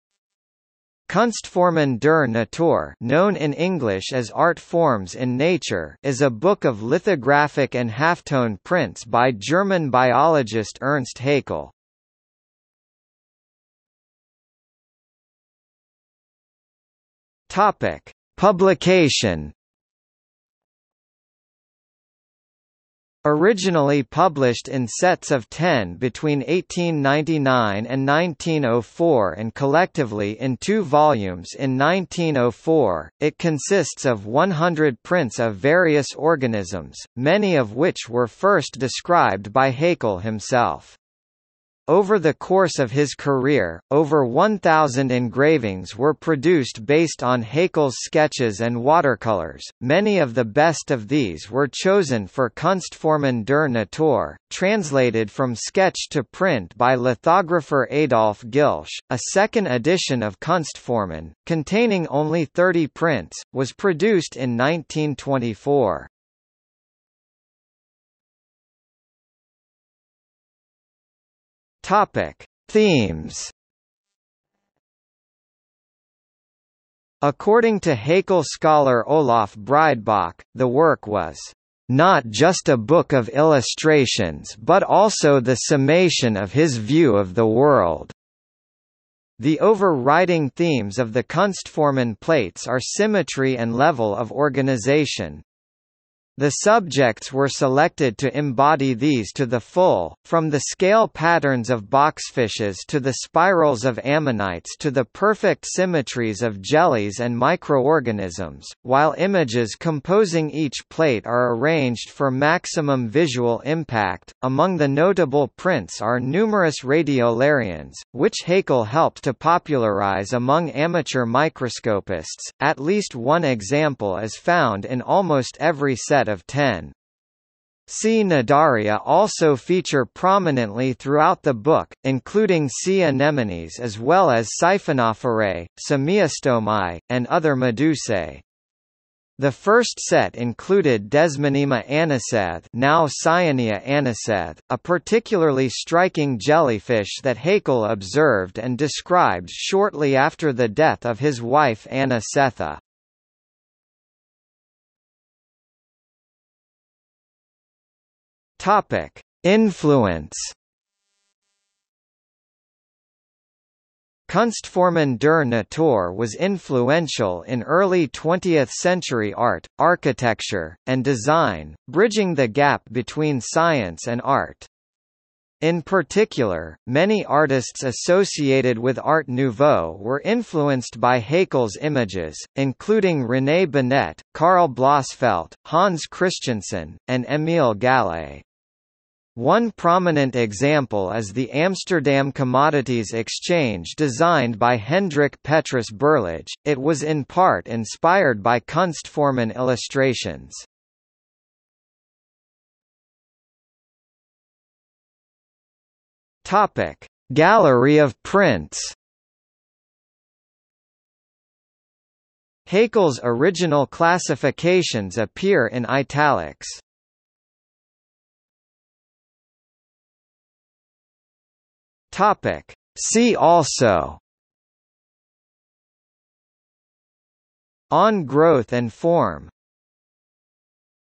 Art Forms of Nature | Wikipedia audio article
00:00:13 1 Publication
00:01:08 2 Themes
00:02:34 3 Influence
00:03:22 4 Gallery of prints
00:03:38 5 See also

- Socrates

SUMMARY
Kunstformen der Natur (known in English as Art Forms in Nature) is a book of lithographic and halftone prints by German biologist Ernst Haeckel.